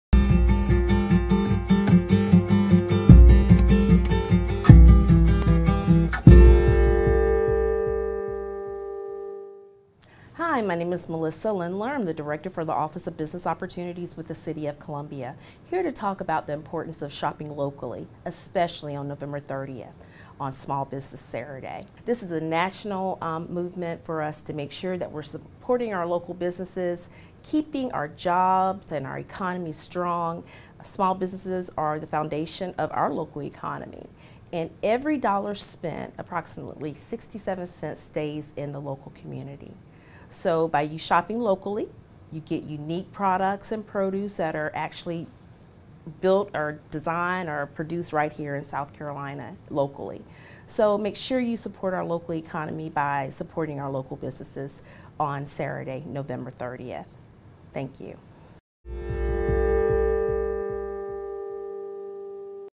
City Vibes | Small Business Saturday
This Saturday, November 30, you can be a part of the 10th annual holiday shopping tradition that helps to bring communities together in support of their
favorite small businesses.

Support local businesses and put money back into the community while getting in that last minute holiday shopping.